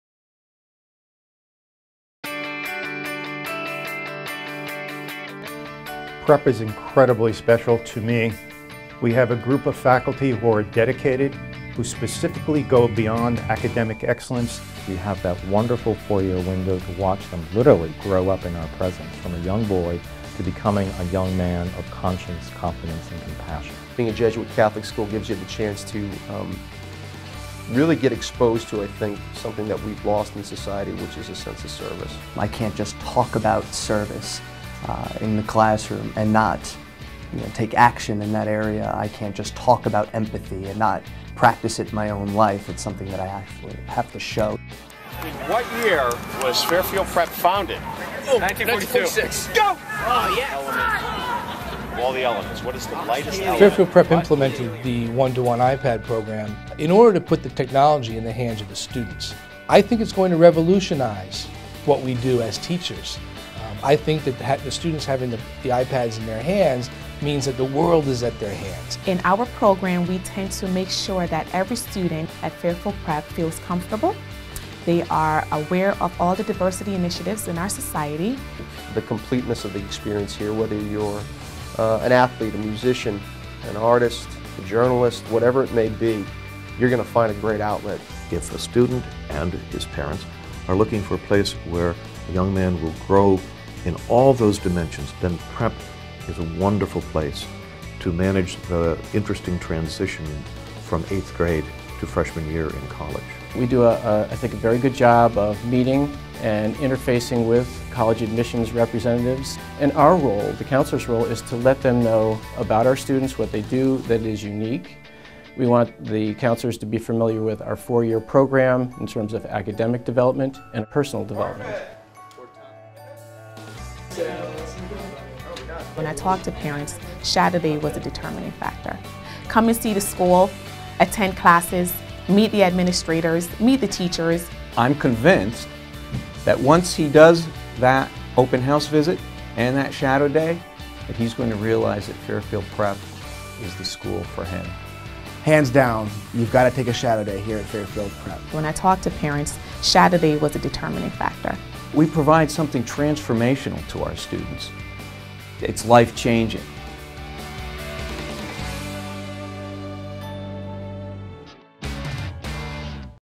Fairfield Prep Faculty & Staff
Fairfield Prep's faculty and staff support the Jesuit, Ignatian methodology of teaching.